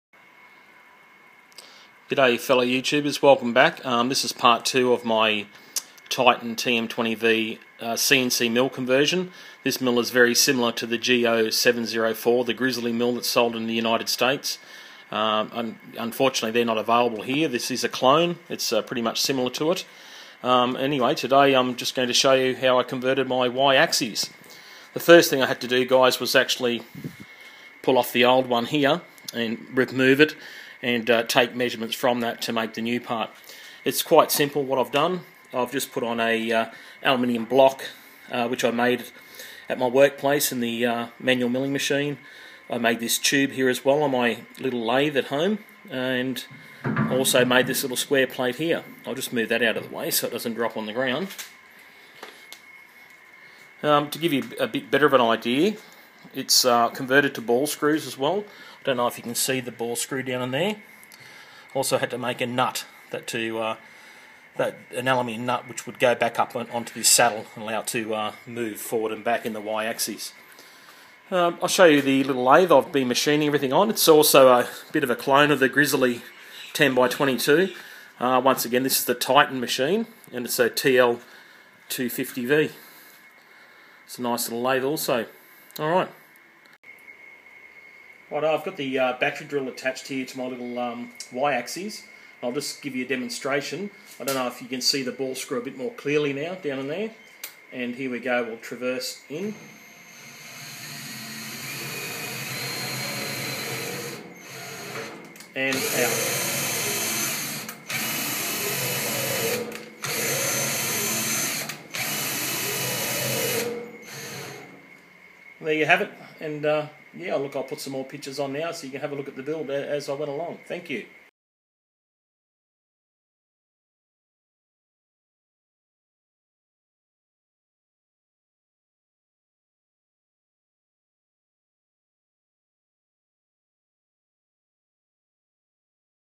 CNC Mill Conversion - Titan TM20LV Part 2
This video is part 2 of a 5 part series that documents the "Y-Axis" conversion of my Titan TM20LV (Grizzly G0704 clone) milling machine  to numerical control.